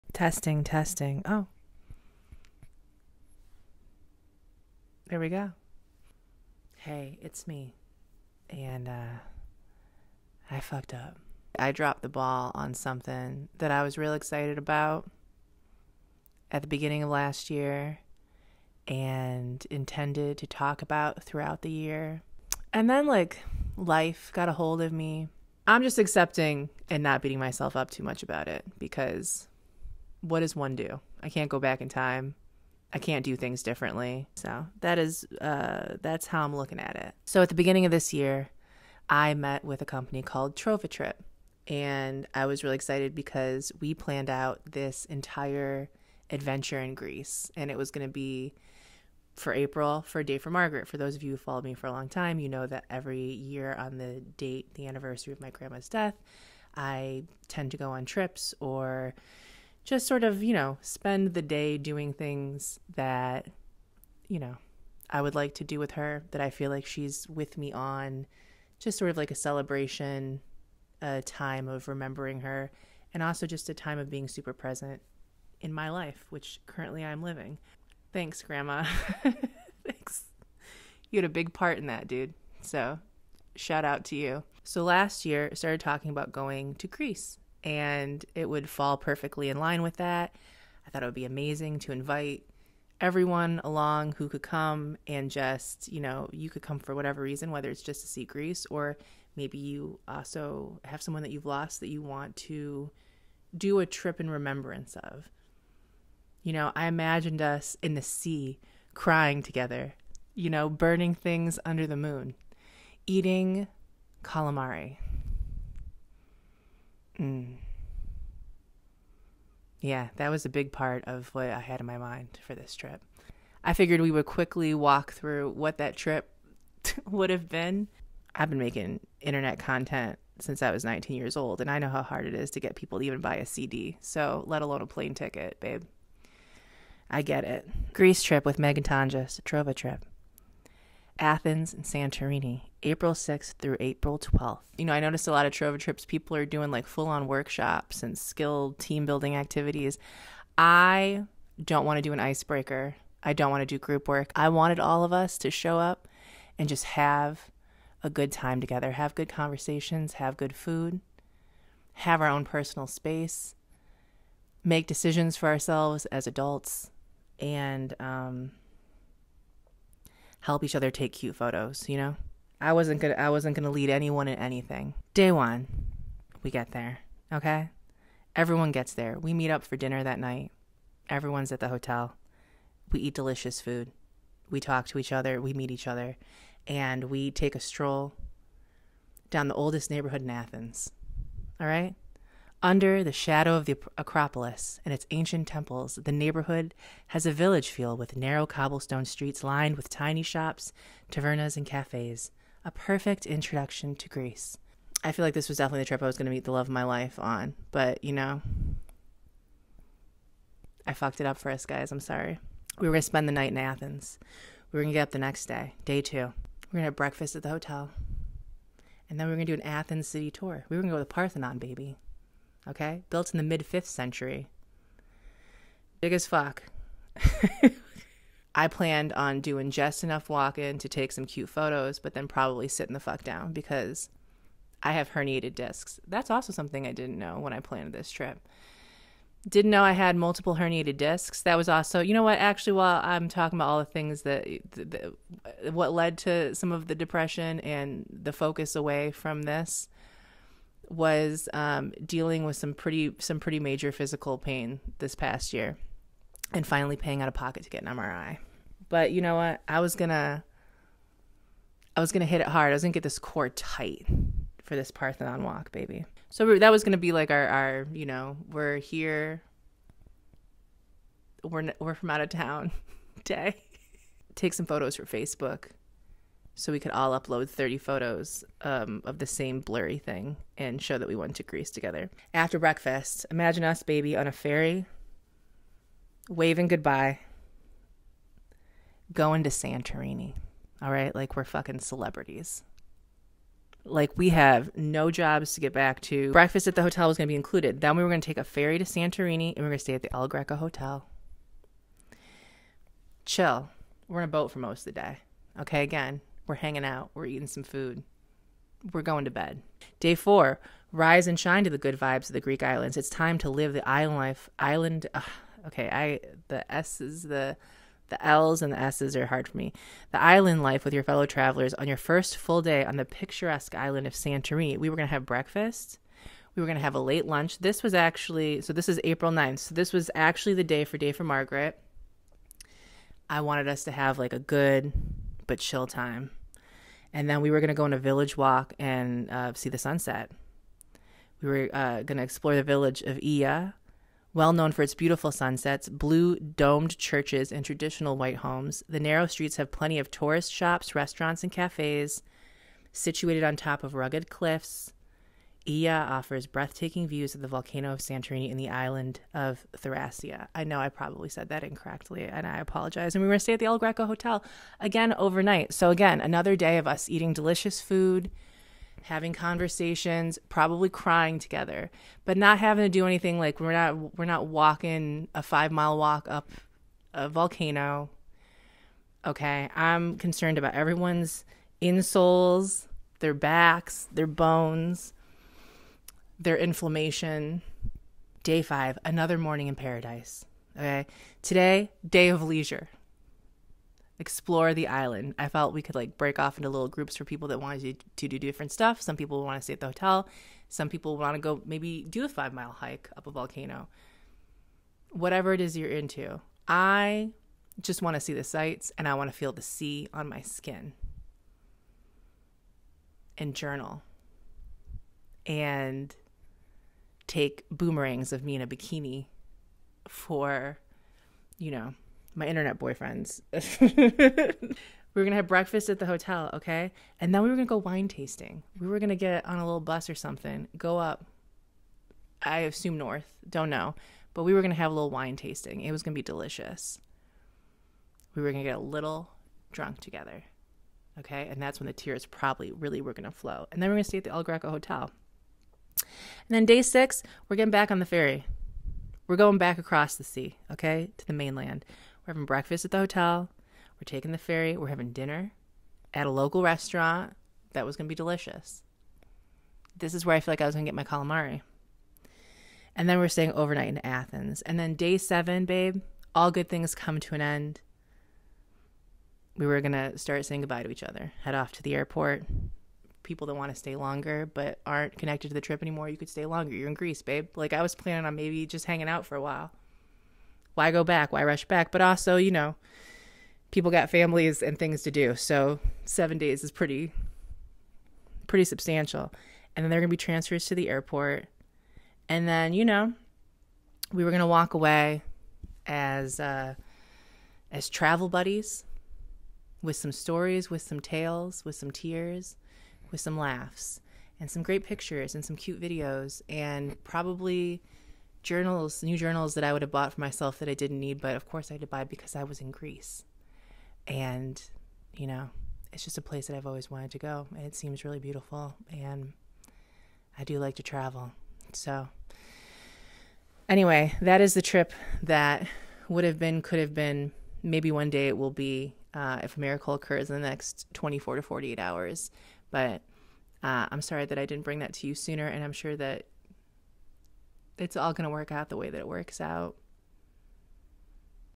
i messed up
It was a good idea in theory!
Sorry Trova!

I talk about the almost Greece trip i was gonna take with TrovaTrip.